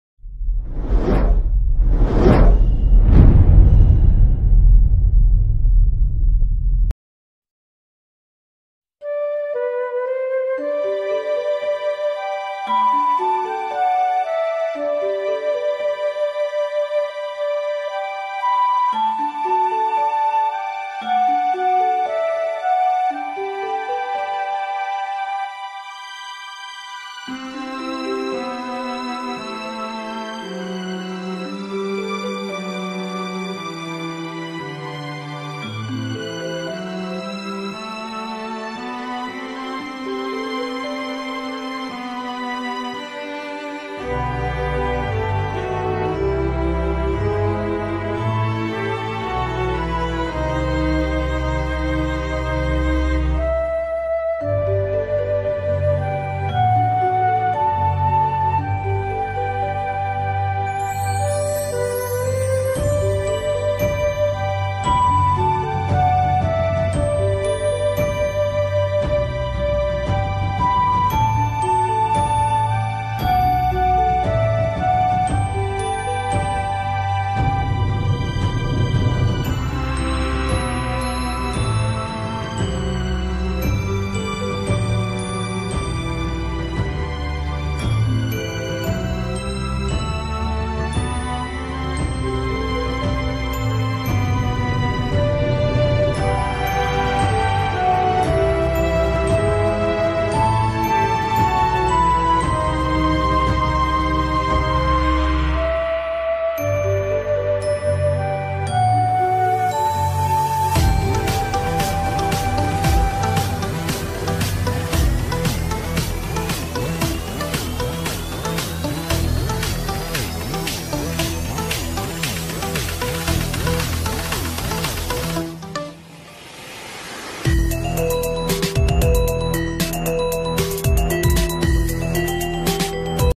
Truk Towing sepesial Kemerdekaan RI 75|| miniatur truk oleng dari kardus
Assalamualaikum guys

Di video kali ini saya akan test oleng truk spesial kemerdekaan RI yang ke 75

Selamat menonton semoga video ini bisa menghibur